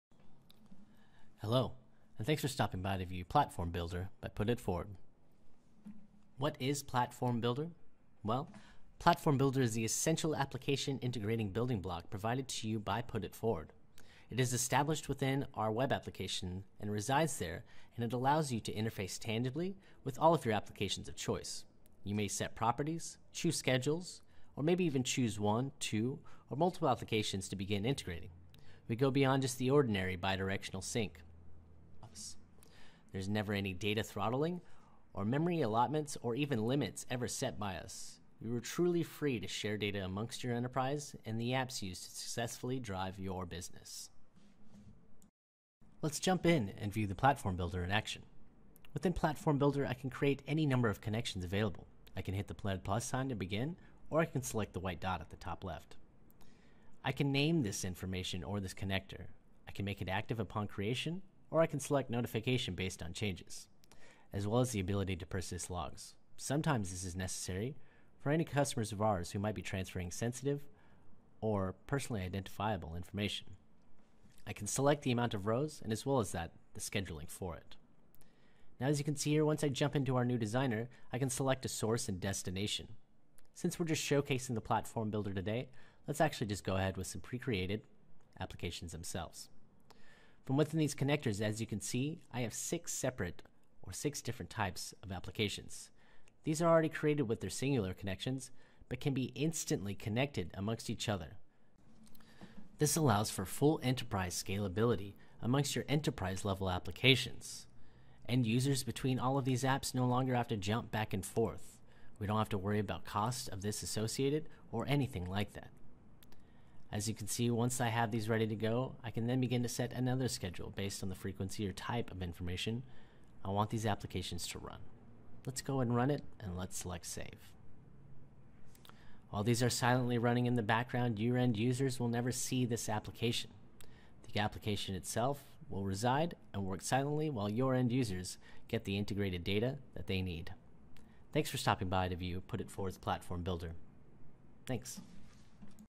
Platform Builder by Put it Forward
Platform Builder is used to link multiple systems together through and orchestrate data and process flows across them.